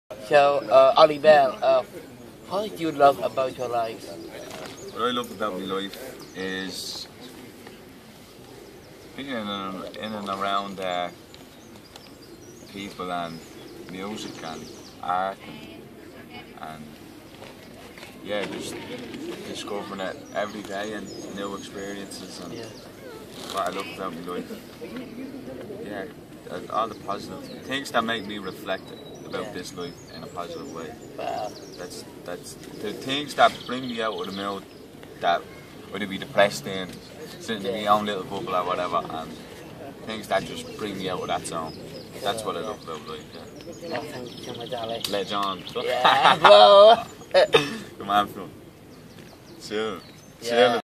What do you love about your life, Ollie Bell?
Fionn interviews Ollie Bell, a singer songwriter from the area of Ballygall, Dublin. His music ranges from blues, jazz, hip hop, soul. He has played both Body & Soul and Electric Picnic.

Recorded at Body & Soul Arts and Music Festival, Ballinlough Castle, Westmeath, June 2016.